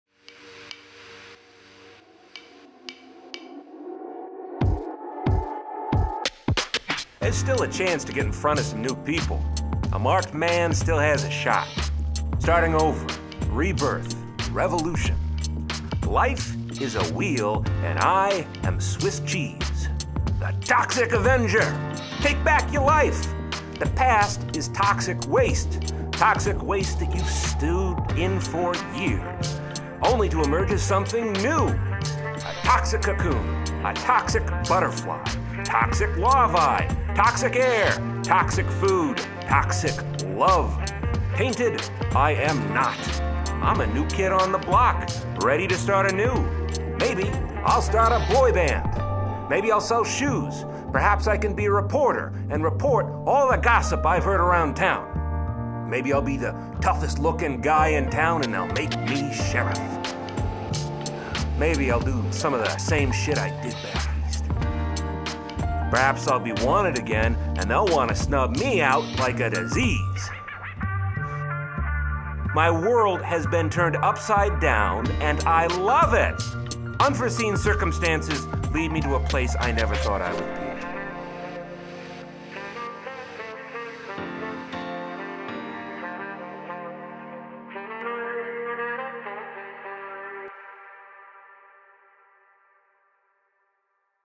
Start Anew
stop motion animation featuring artwork/words by brent houzenga